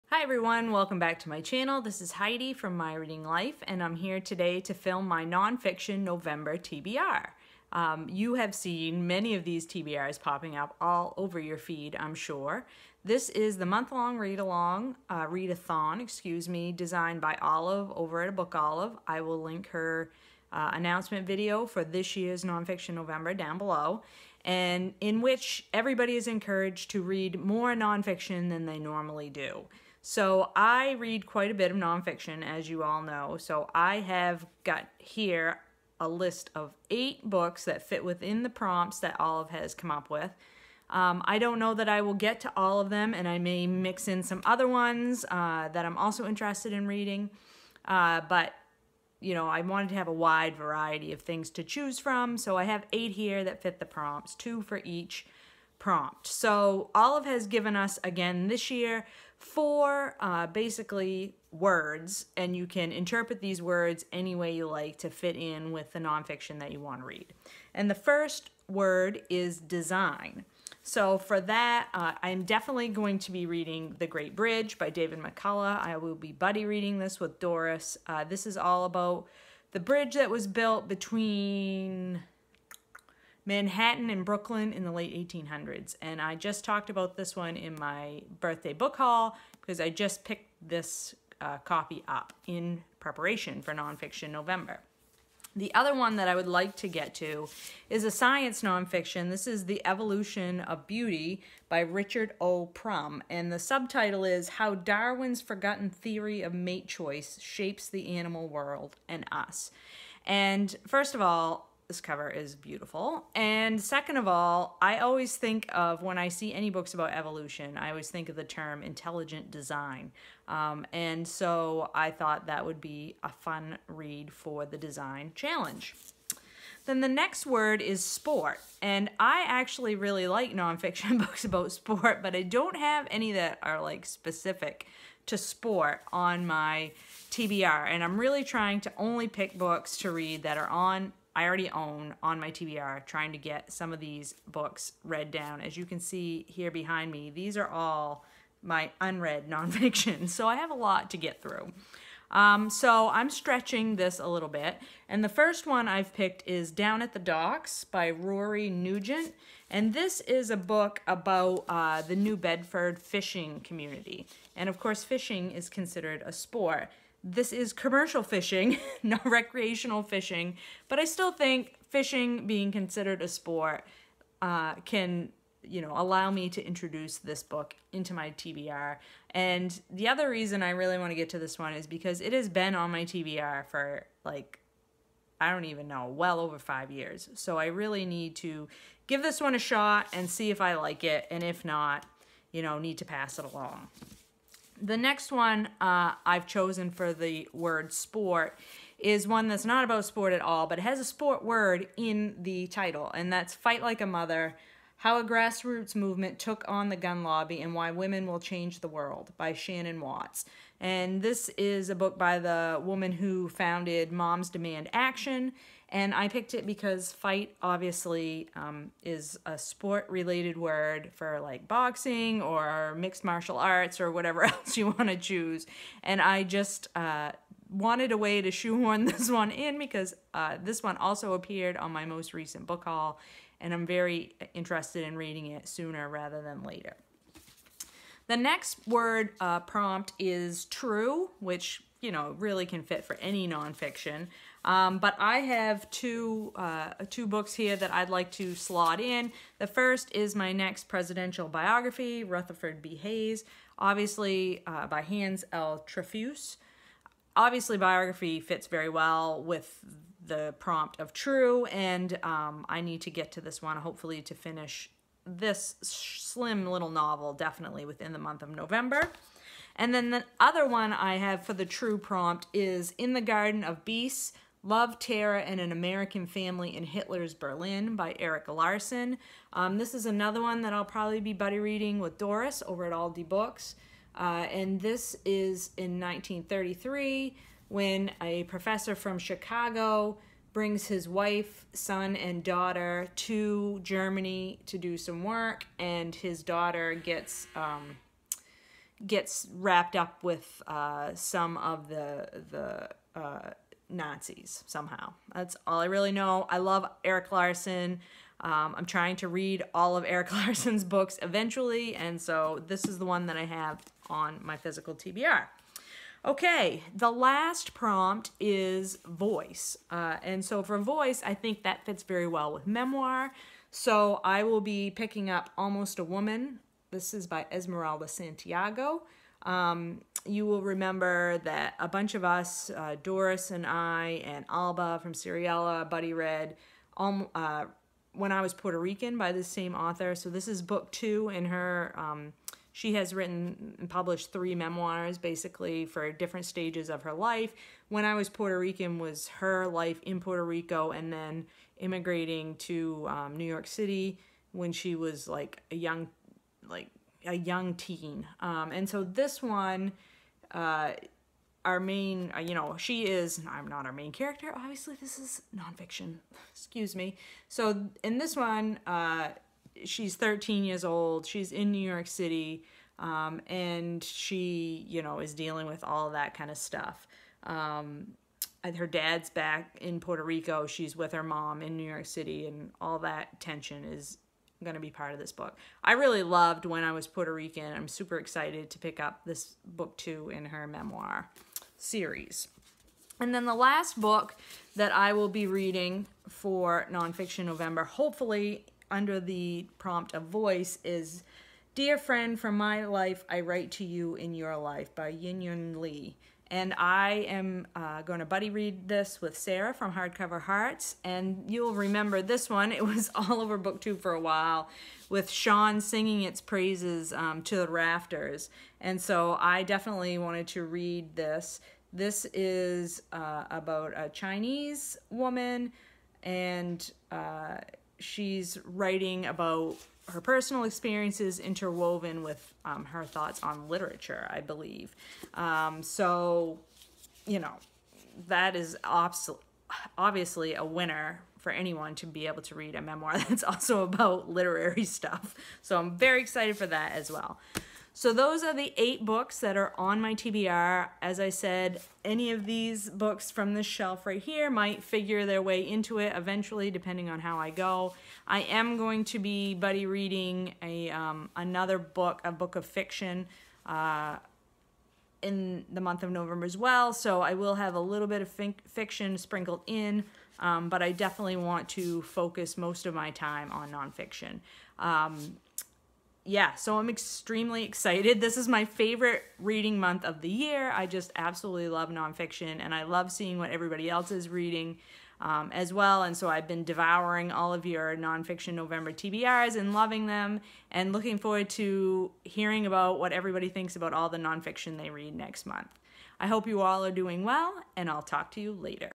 Nonfiction November TBR
Books mentioned:
The Great Bridge by David McCullough
The Evolution of Beauty by Richard O. Prum
Down at the Docks by Rory Nugent
Fight Like a Mother by Shannon Watts
Rutherford B. Hayes by Hans L. Trefousse
Almost A Woman by Emeralda Santiago
In the Garden of Beasts by Erik Larson
Dear Friend, From My Life I Write to You in Your Life by Yiyun Li